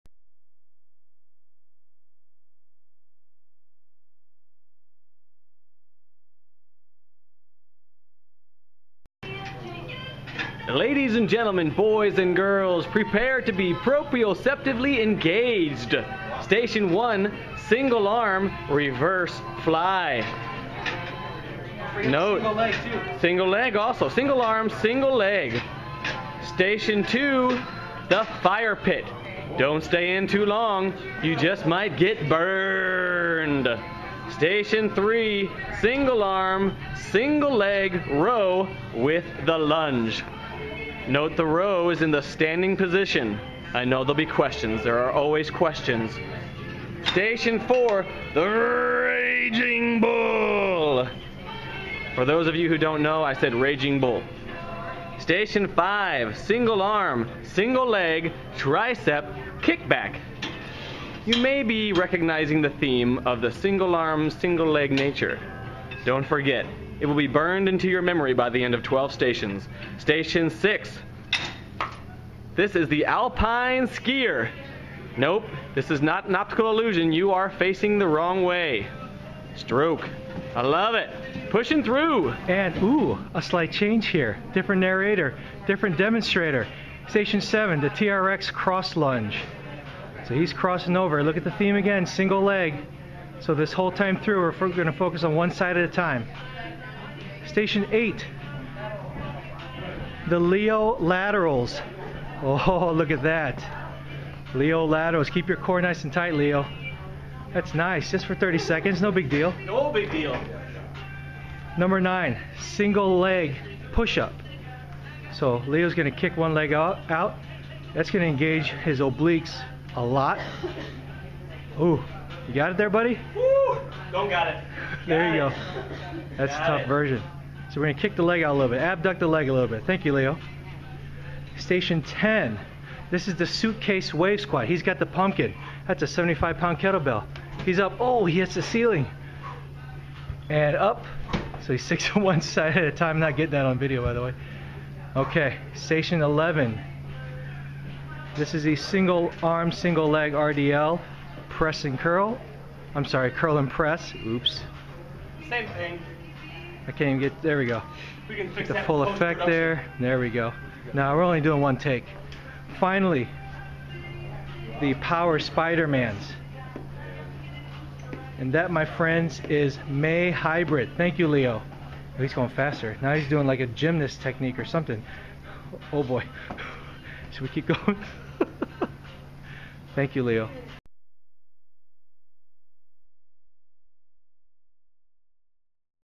NutriFormance Hybrid Group Training - May 2012
Metabolic Strength circuits for groups of up to 12 people.